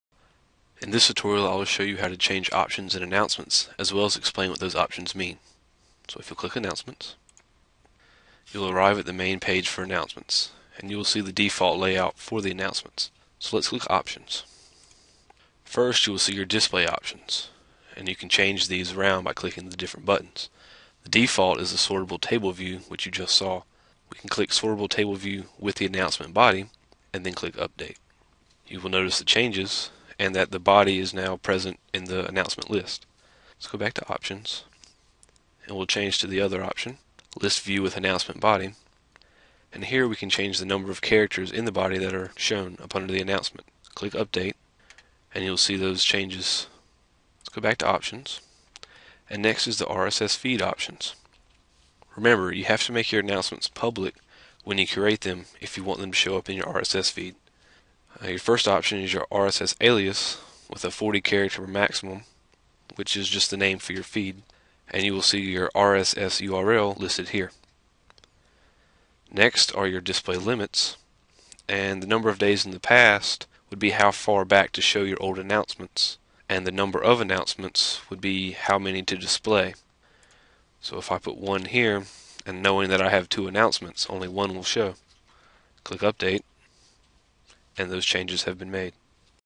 Sakai Announcements: Change display
Learn what options are available in the Announcements tool.